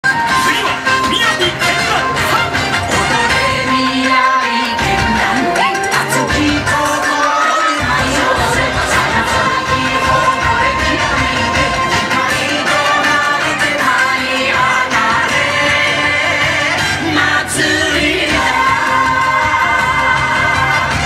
TDL 雅涼群舞 8/2 1回目 ミッキーの投げチュウ
2015/8/2　1回目公演

ミッキーのカッコいい投げチュウです
この頃はまだミッキーだけでやってたんですよね・・・